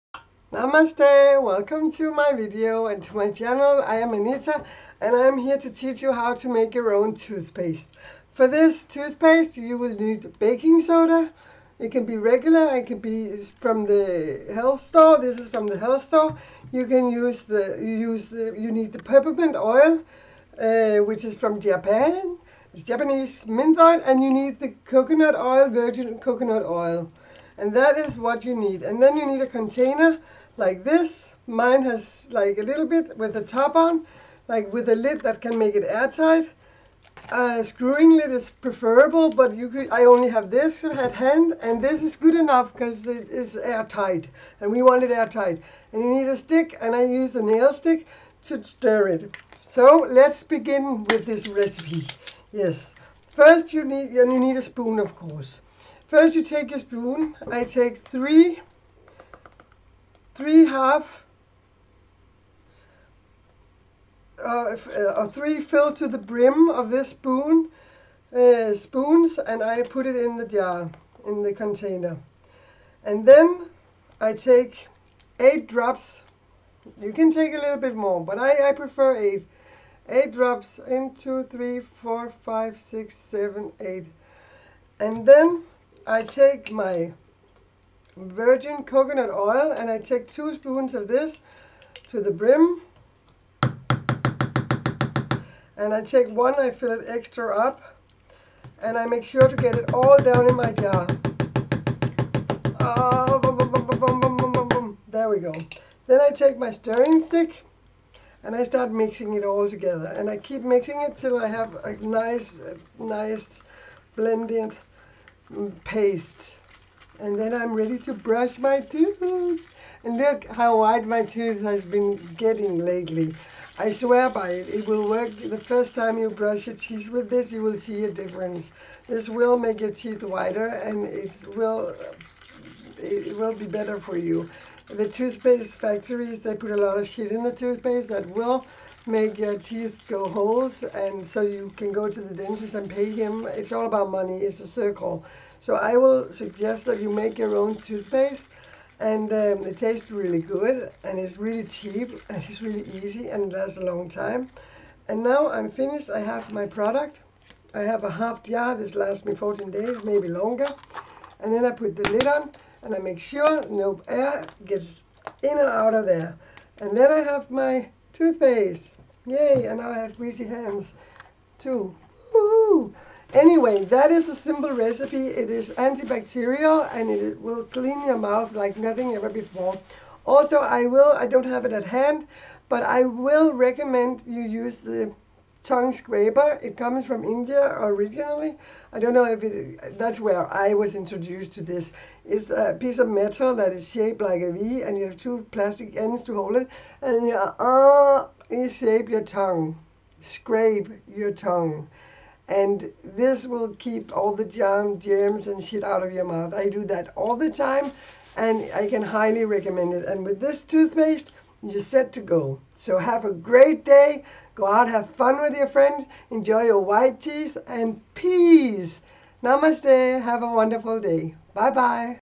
DIY toothpaste
how u fast make A super good whitening toothpasta antibacterial :)